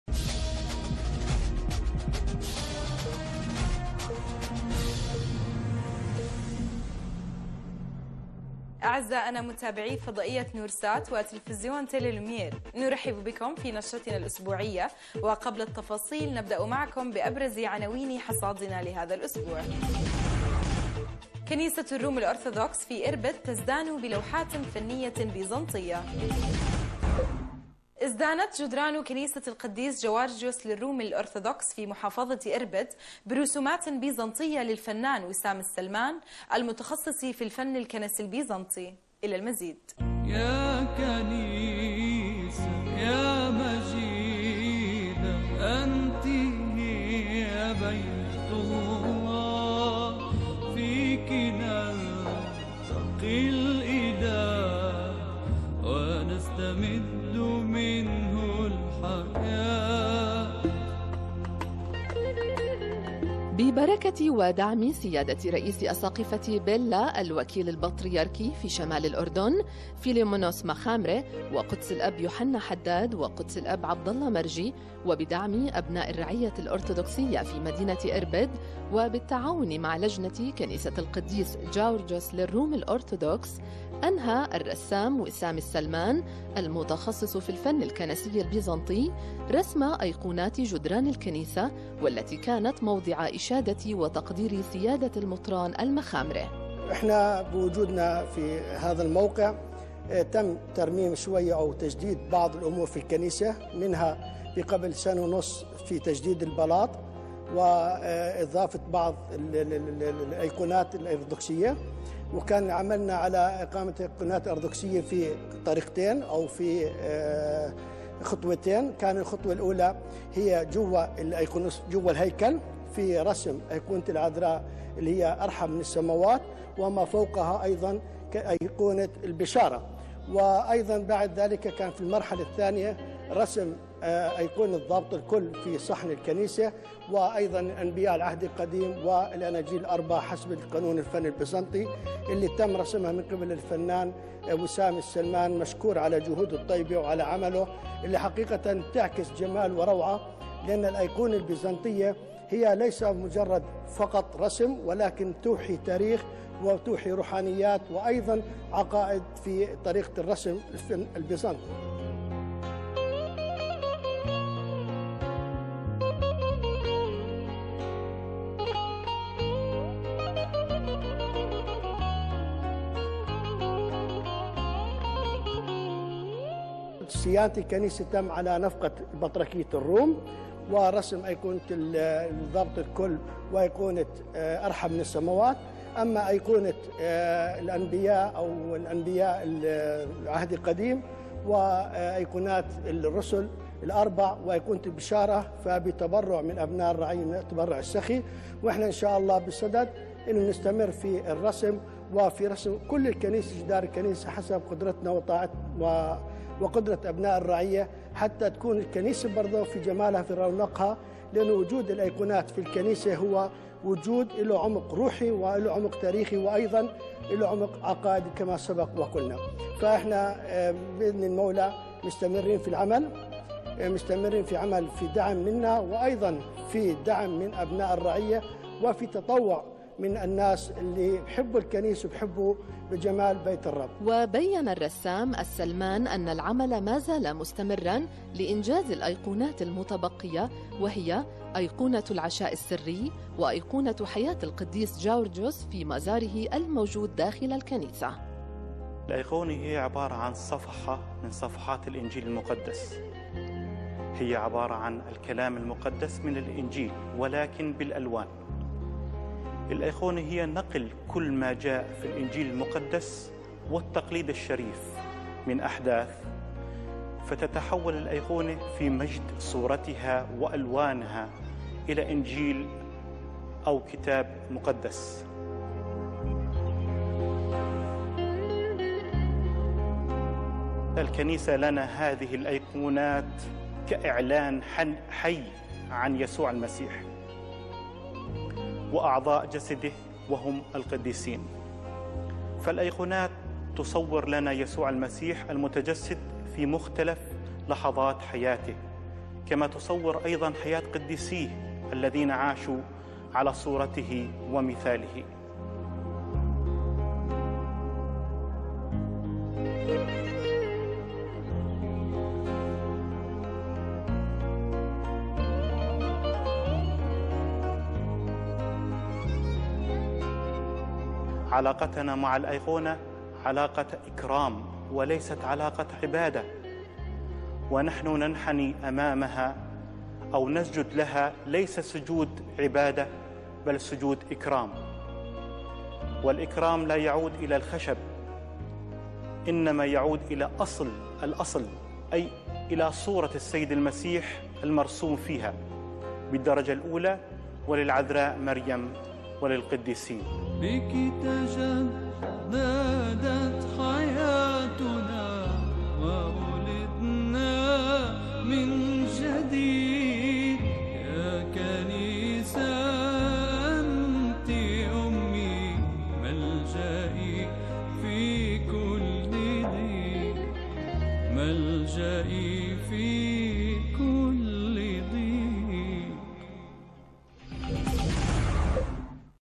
تقرير الرسم البيزنطي من نورسات 15/7/2016
تقرير قناة نورسات حول الرسم البيزنطي للفنان وسام السلمان
 من كنيسة القديس جيوارجيوس اربد حول رسم الكنيسه
15/7/2016